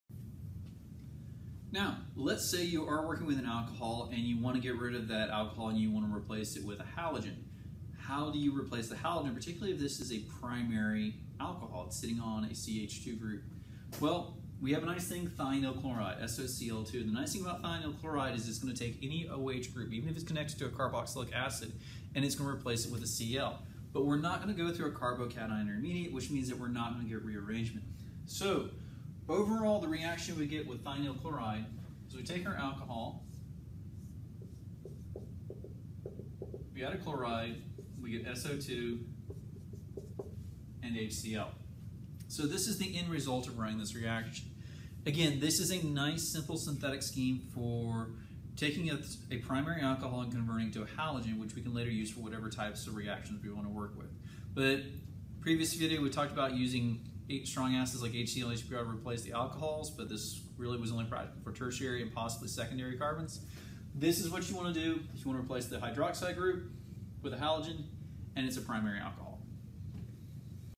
Thionyl Chloride and Primary Alcohols
Here we go through what thionyl chloride can do for you